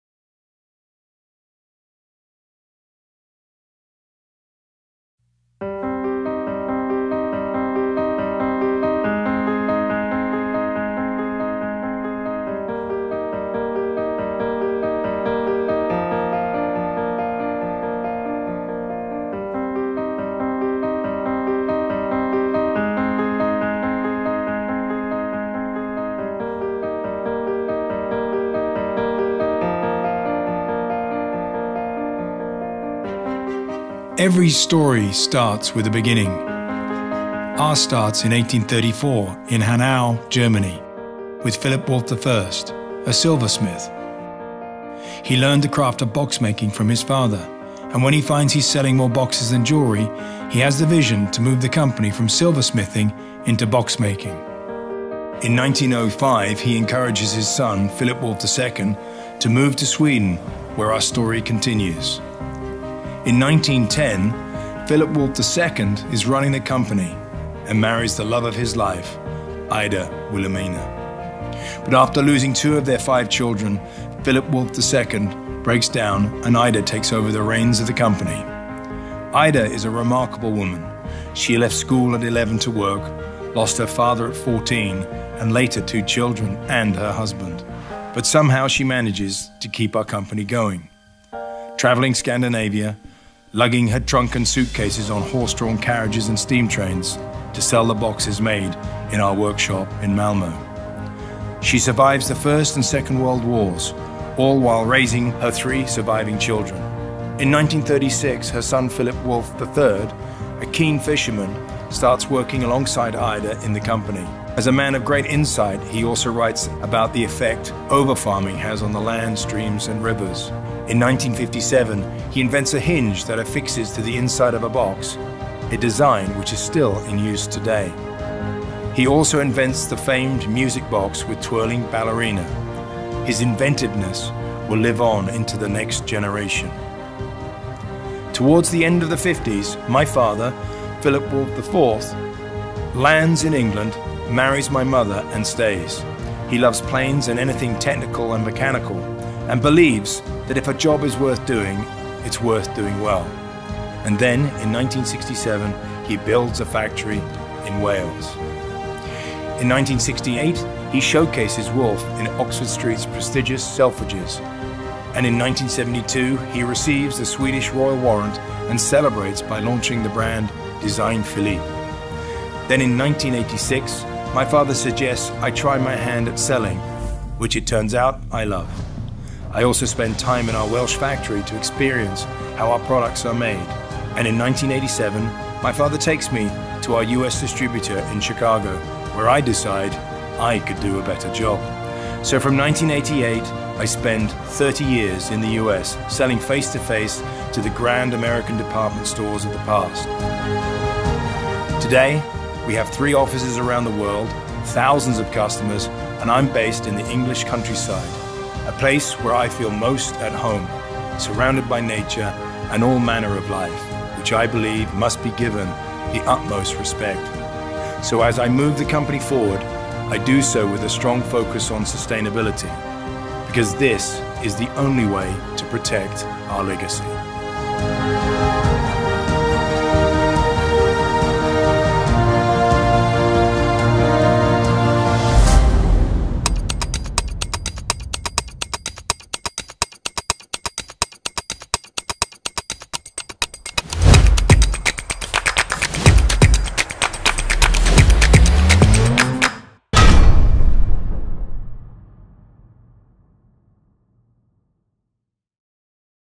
History of the creation of the Wolf brand - 190 years of tradition • Watchard.com
Wolf was founded in 1834. Over 190 years, it has gained stus of a luxury brand with global reach. Learn about the remarkable story of a family business that brought generations together.